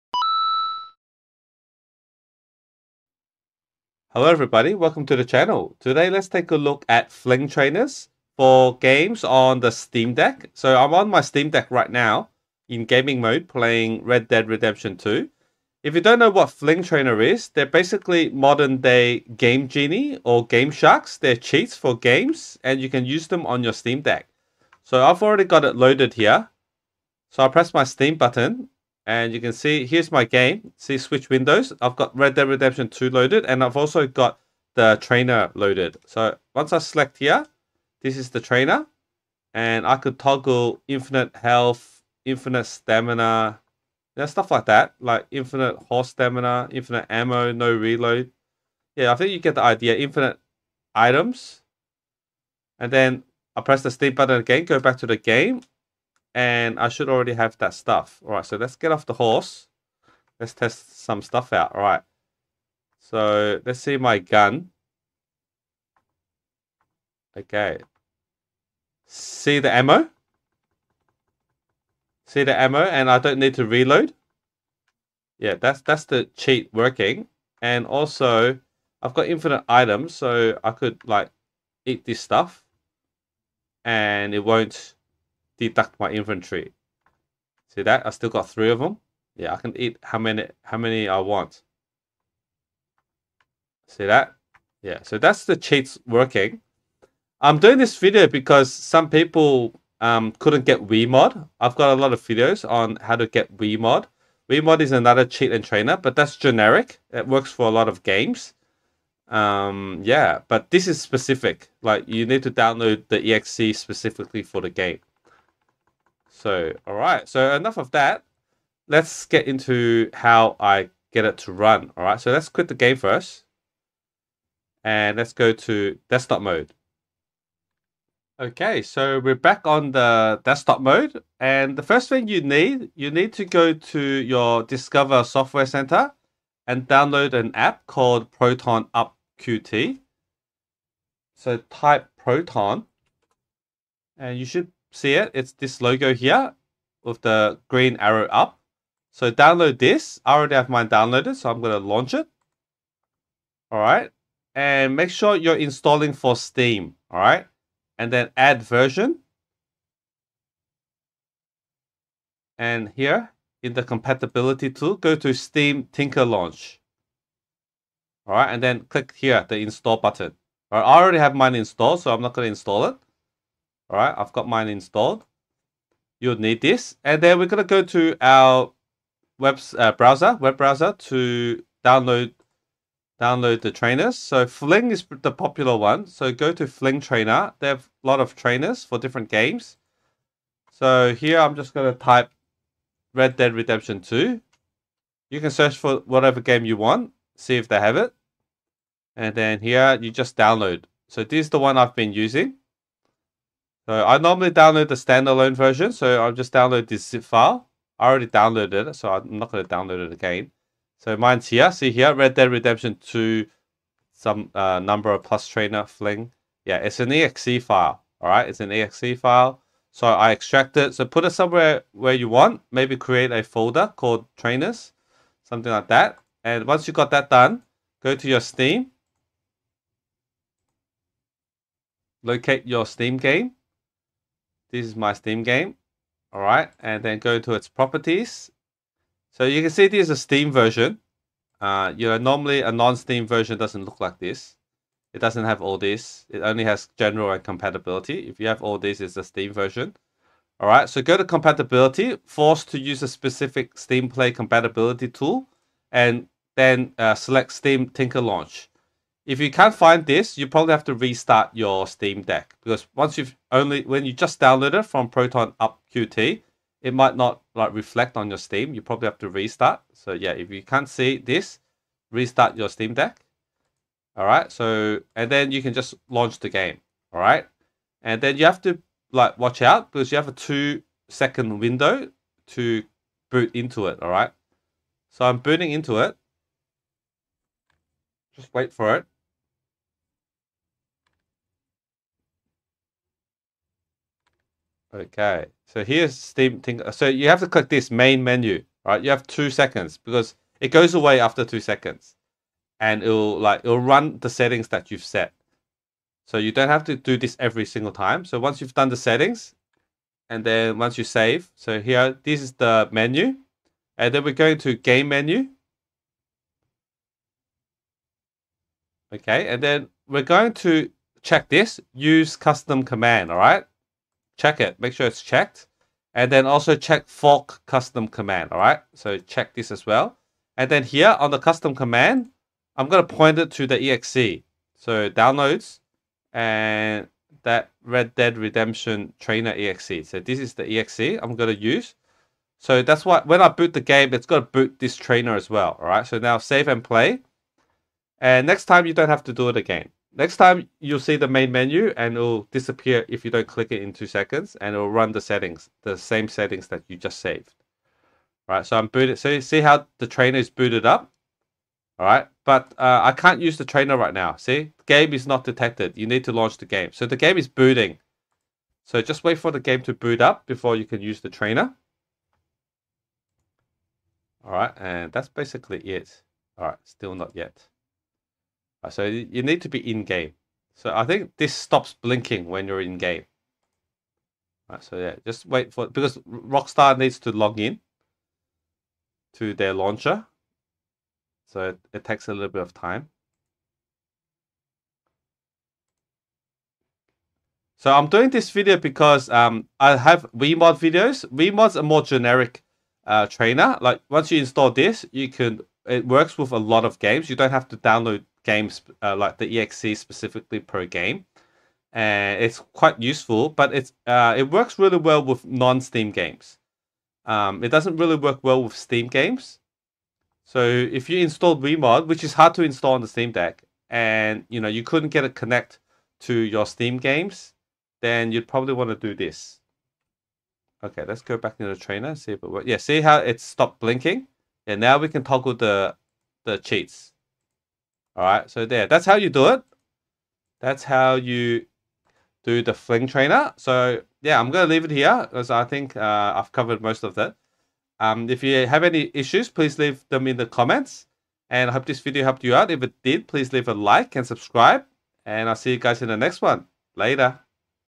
Steam Deck FLiNG Trainer Tutorial | Cheat your games on Steam Deck
This video demonstrates how to use FLiNG Trainer on Steam Deck.